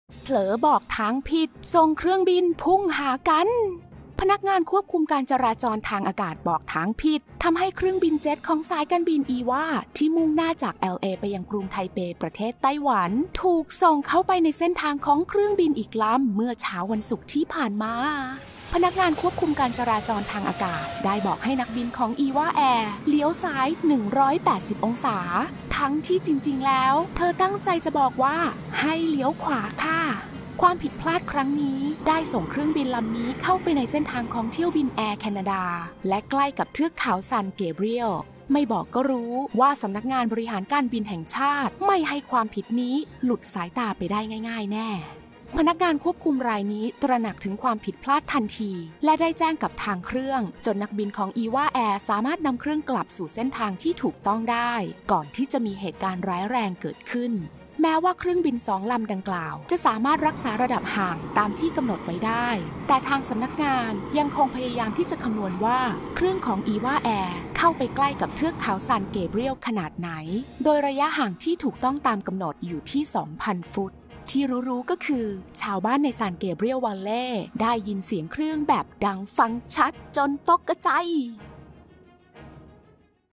เผลอบอกทางผิด ส่งเครื่องบินพุ่งหากัน
LOS ANGELES  —  เครื่องบินเจ็ทของสายการบิน EVA ที่มุ่งหน้าจากแอลเอไปยังกรุงไทเป ประเทศไต้หวัน ถูกส่งเข้าไปในเส้นทางของเครื่องบินอีกลำเมื่อเช้าวันศุกร์ที่ผ่านมา

พนักงานควบคุมการจราจรทางอากาศได้บอกให้นักบิน EVA เพื่อให้เลี้ยวซ้าย 180 องศา ทั้งที่เธอตั้งใจจะบอกให้เลี้ยวขวา ความผิดพลาดนี้ได้ส่งเครื่องบินลำนี้เข้าไปในเส้นทางของเที่ยวบินแอร์แคนาดา และใกล้กับเทือกเขาซานเกเบรียล

พนักงานควบคุมรายนี้ตระหนักถึงความผิดพลาด และได้แจ้งกับทางเครื่อง จนนักบินสามารถนำเครื่องกลับสู่เส้นทางที่ถูกต้องได้ทัน ก่อนที่จะมีเหตุร้ายแรงเกิดขึ้น

เครื่องบินสองลำดังกล่าวรักษาระดับห่างตามที่สำนักงานบริหารการบินแห่งชาติกำหนด  อย่างไรก็ตามนักวิจัยด้านการบิน ยังคงพยายามที่จะคำนวนว่าเครื่องของ EVA เขาไปใกล้เทือกเขาซานเกเบรียลขนาดไหน โดยระยะห่างที่ถูกต้องตามกำหนดอยู่ที่ 2,000 ฟุต